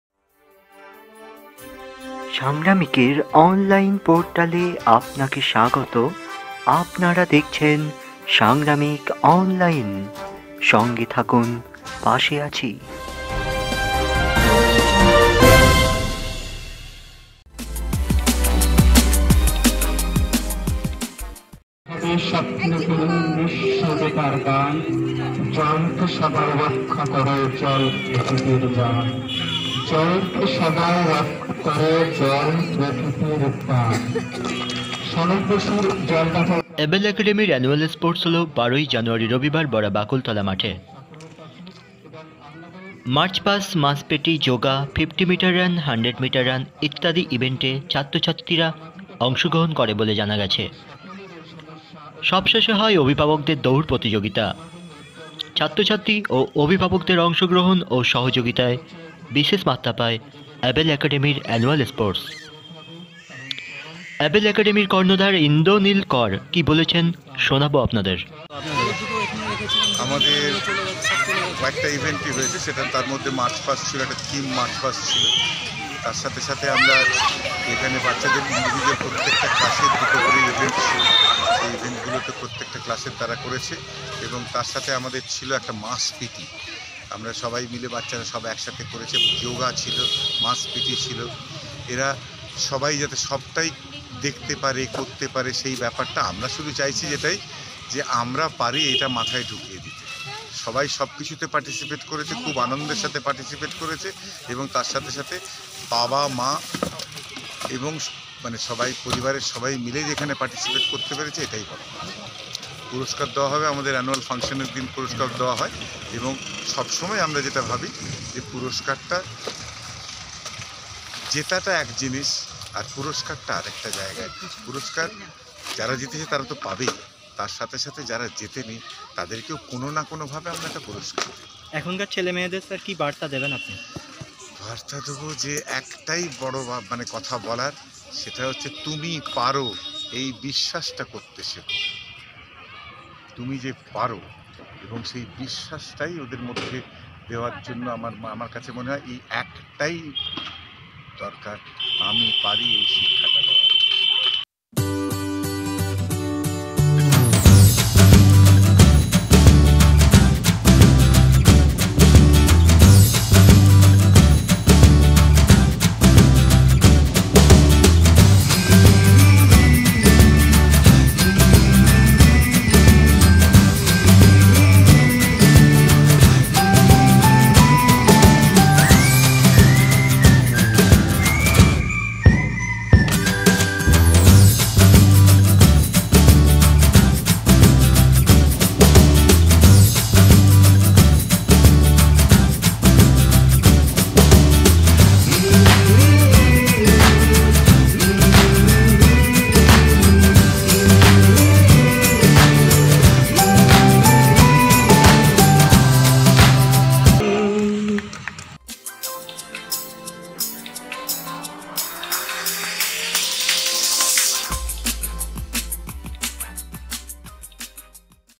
এবল অ্যাকাডেমীর অ্যানুয়াল স্পোর্টস।Annual sports of Able Academy.
Annual sports of Able Academy at Bora, bakaltala.
চ্যানেলে সম্প্রচারিত বক্তব্য বক্তার নিজস্ব, চ্যানেল কর্তৃপক্ষ কোনভাবেই দায়ী নয়।